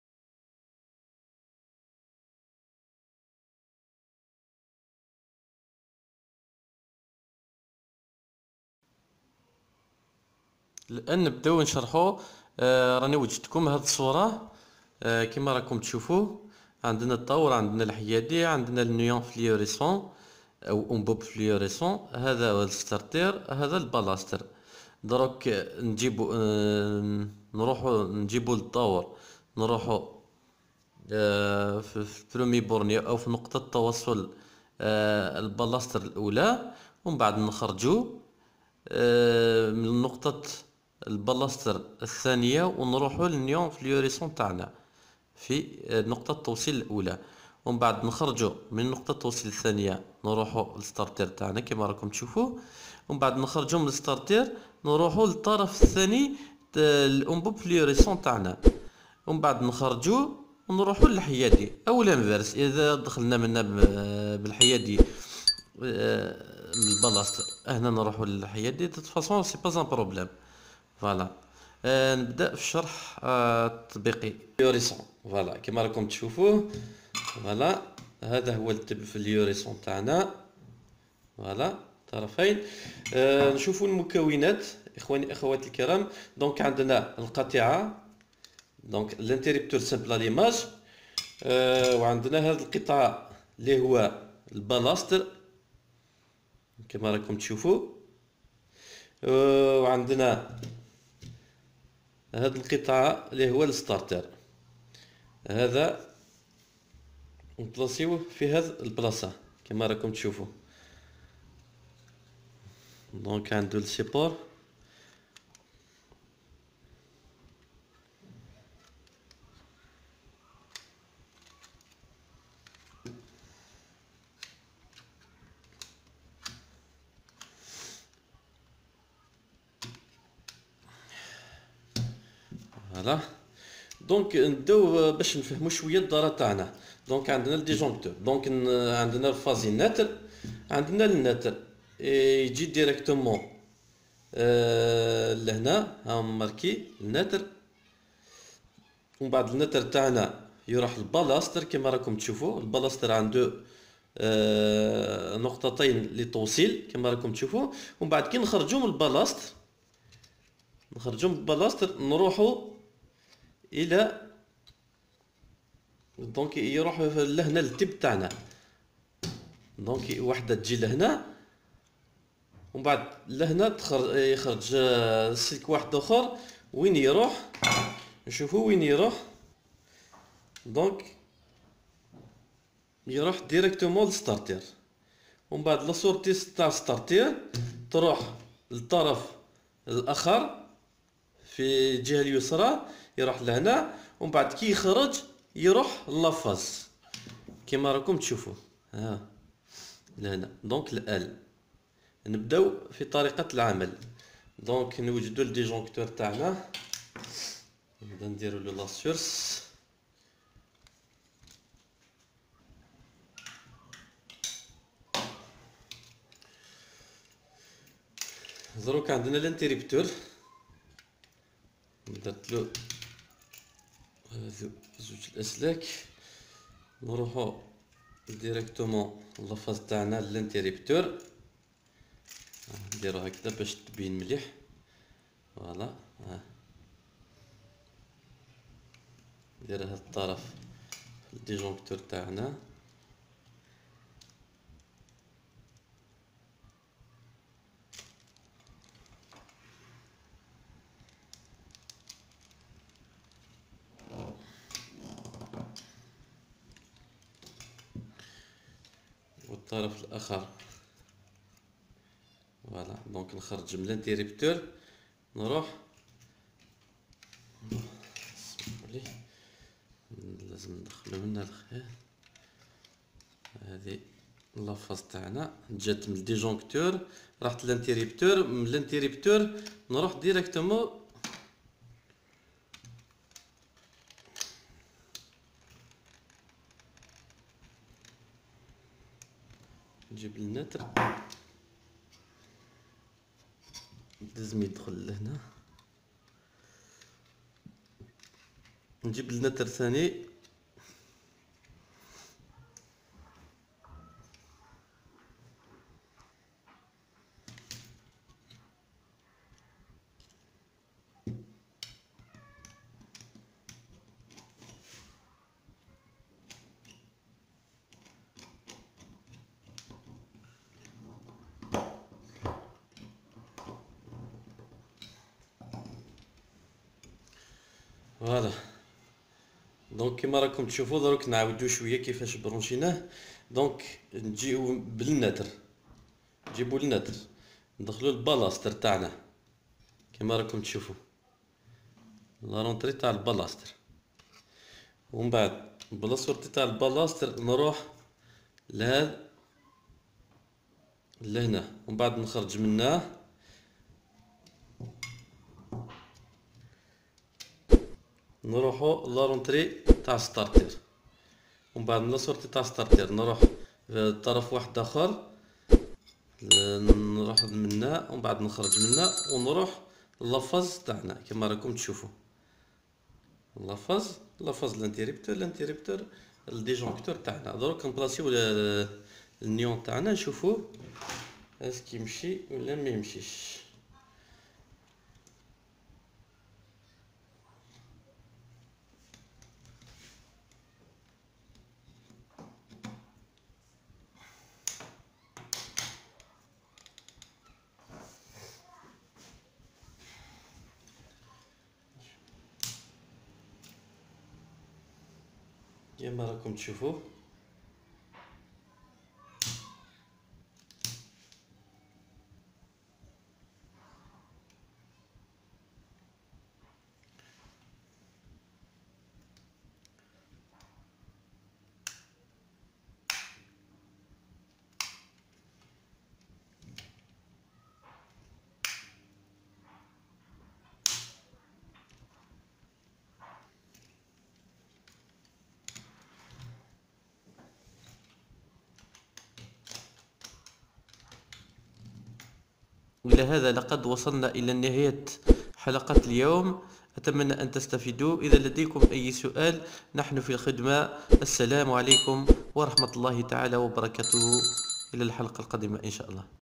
شرح كيفية توصيل الانبوب الفلورسنت
في هده الحلقة اشرح كيفية التوصيل او تركيب الفلورسنت